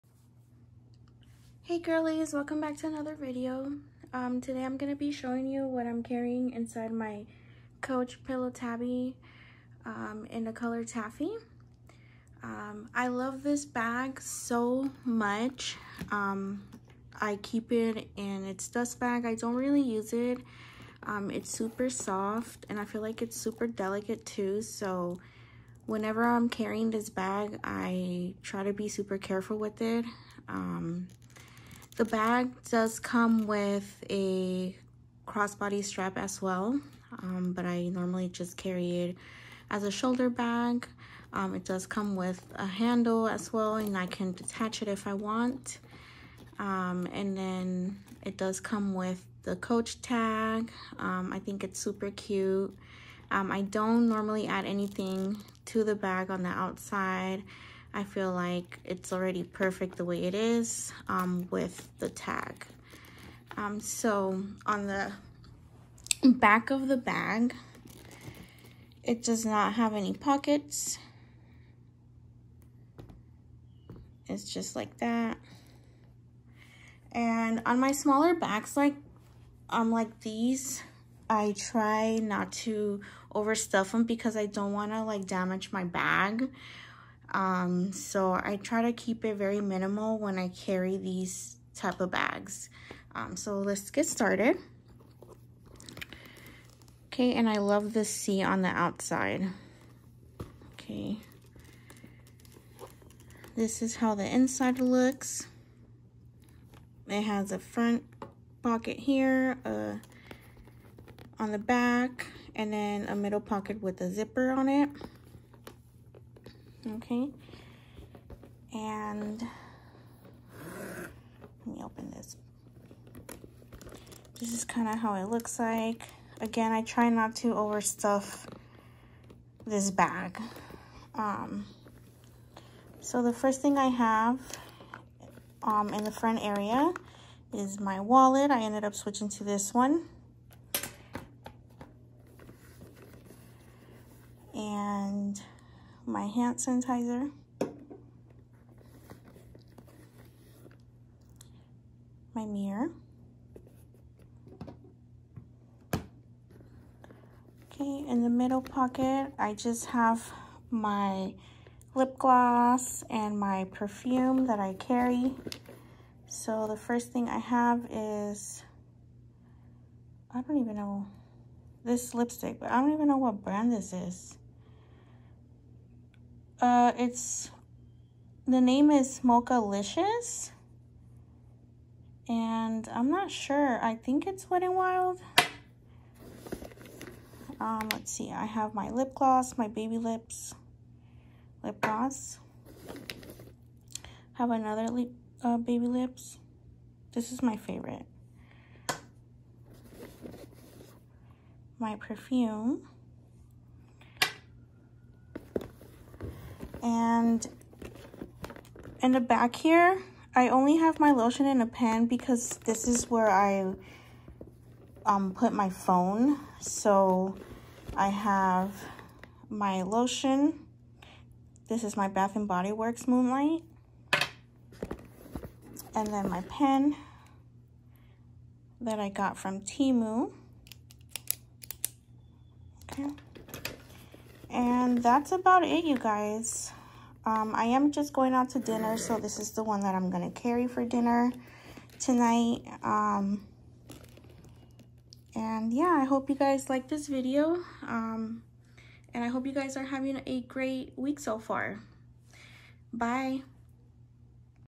What’s in my Coach Pillow Tabby26 💗💗coach
Hello girlyyz if you guys like my bag @Coach did a price drop on this bag . I just checked this morning 💕😊